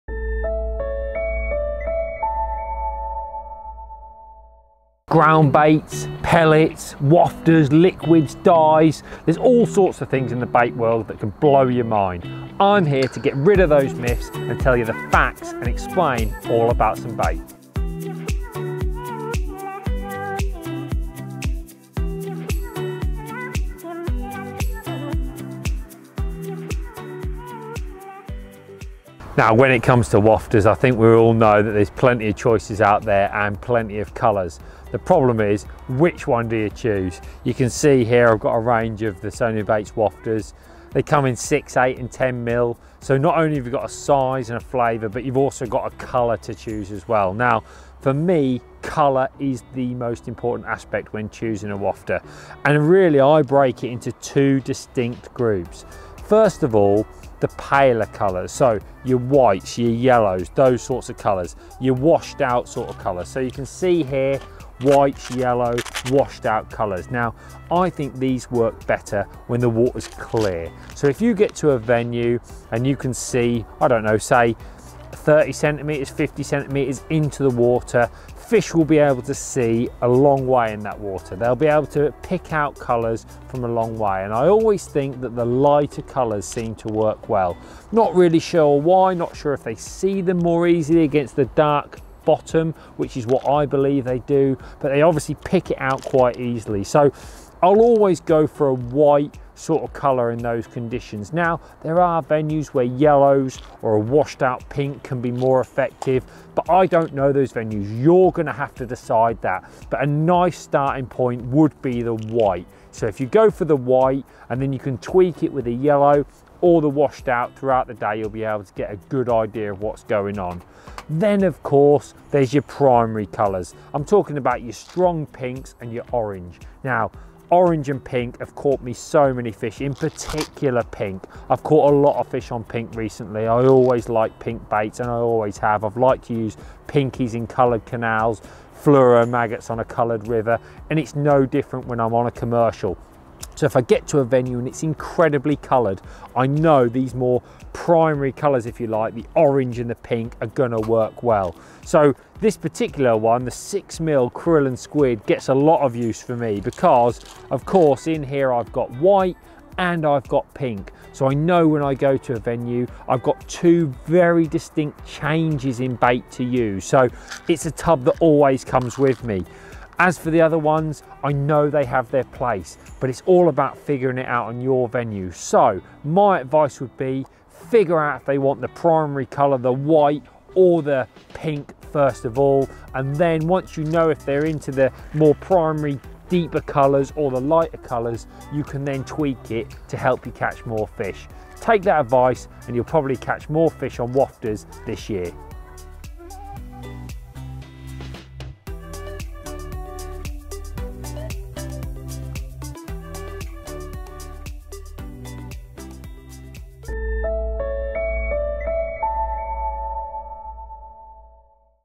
Lee Kerry Explains | Band'Um Wafters
In part IV of his new bait-related video series England international Lee Kerry explains when and where certain colours of wafter come into play in his own fishing. Follow his expert advice and catch more fish in the coming months… 🎣